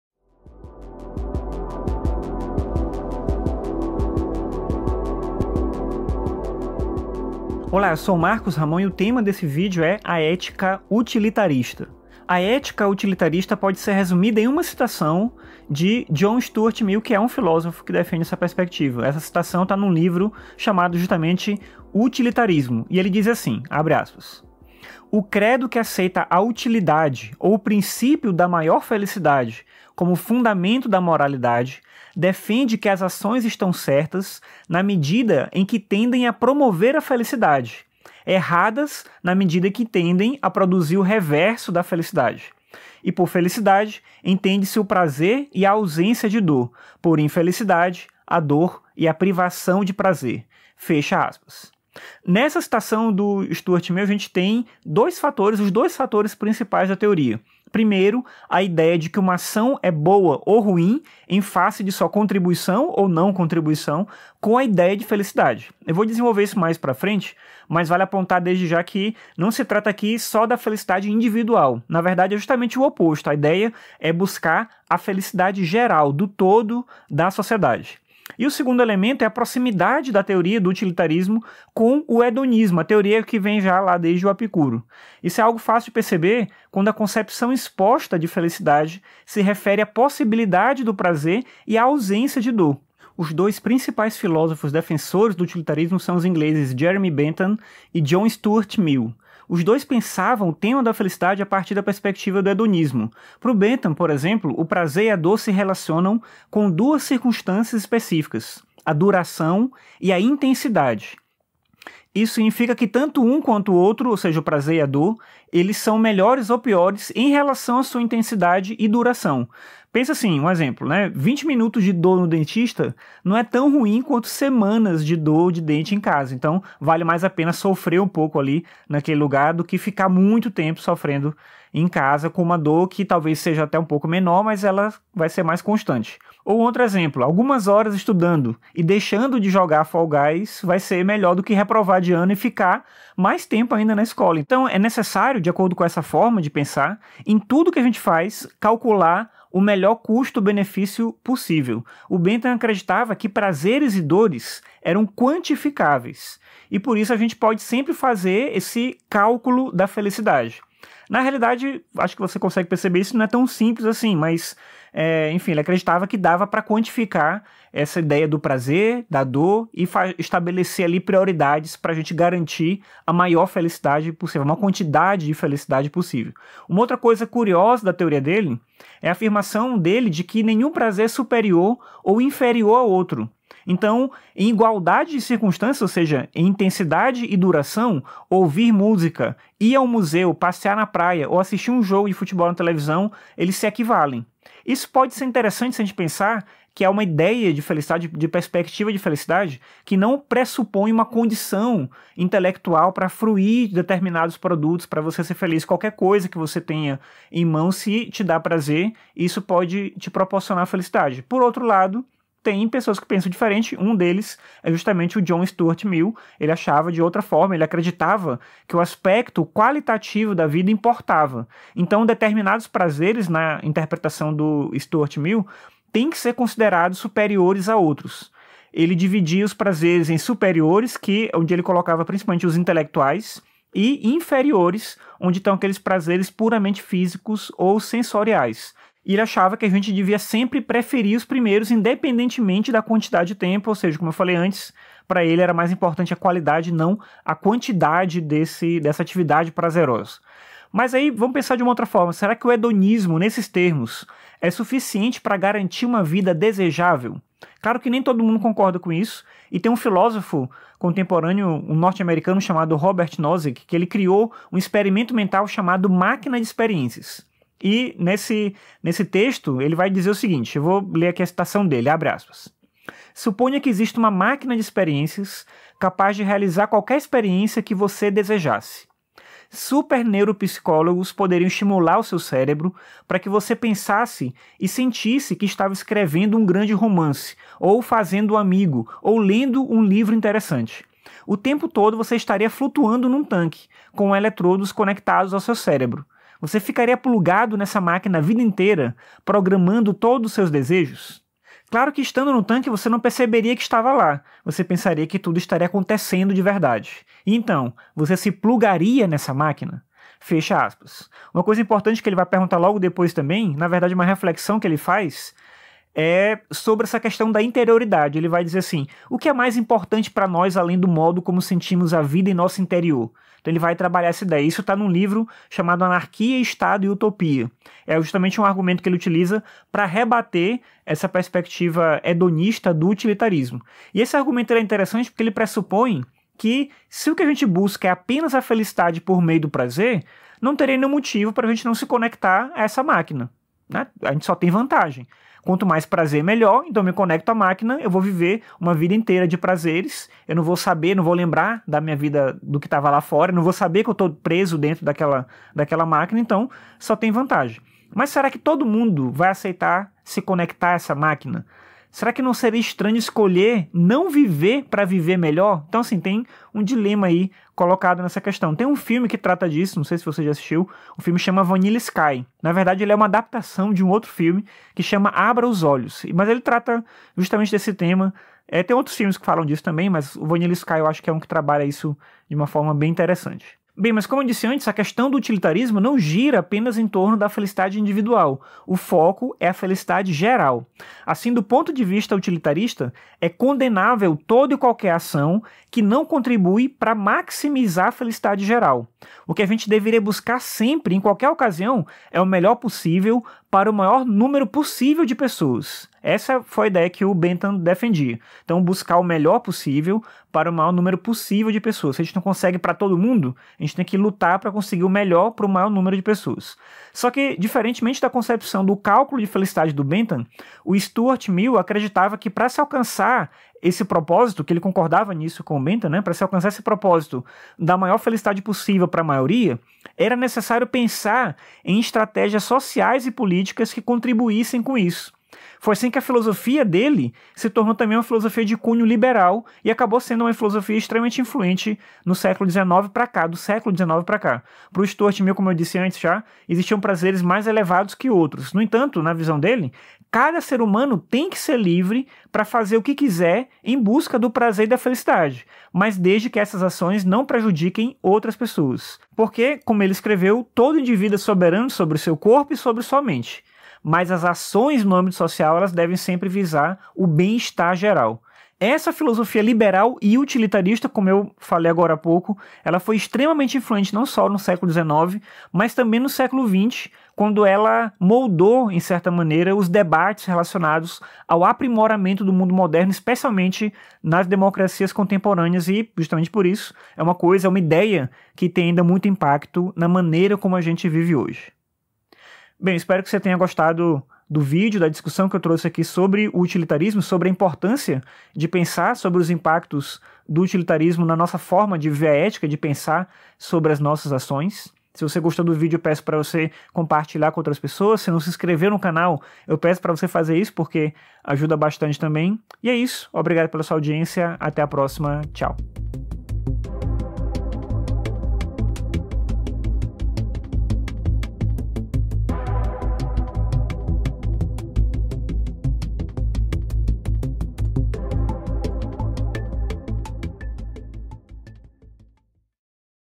A ética utilitarista
Nesse vídeo sobre o aspecto ético do utilitarismo, a partir das discussões propostas por Jeremy Bentham e John Stuart Mill.